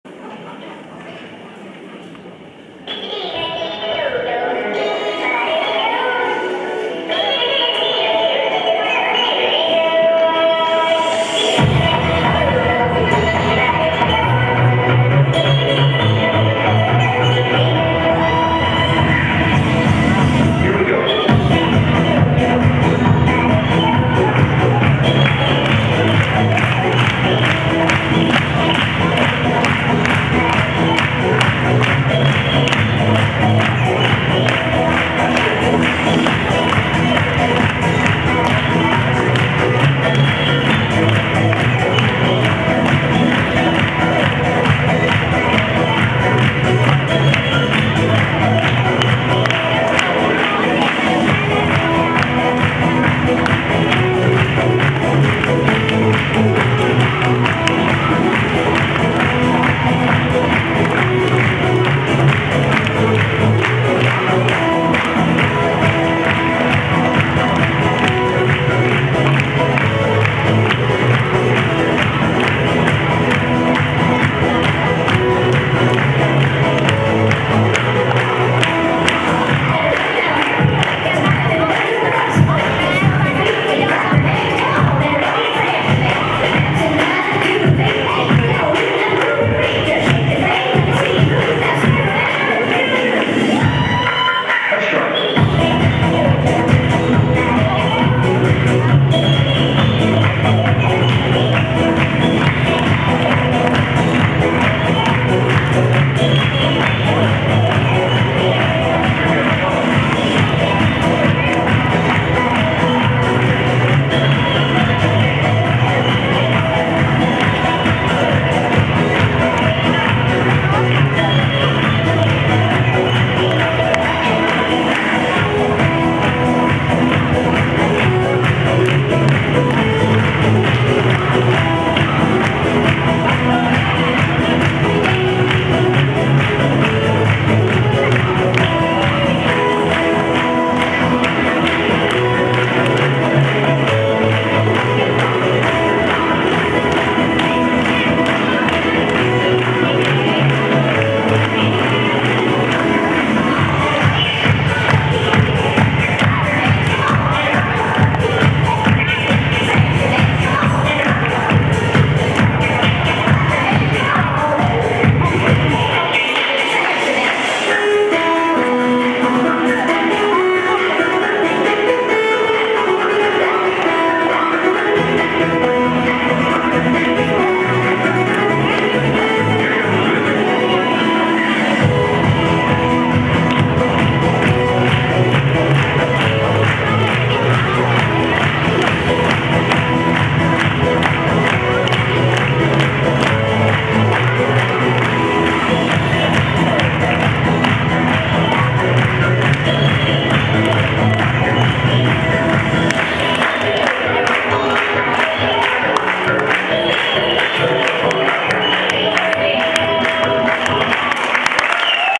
Ceruzatánc
Rózsa úti Általános Iskola 1.a osztályának farsangi műsora